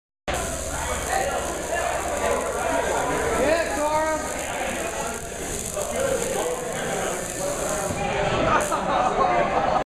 Shocknife overhand stab defense
Sara vs Ryan overhand stab defense with shocknife.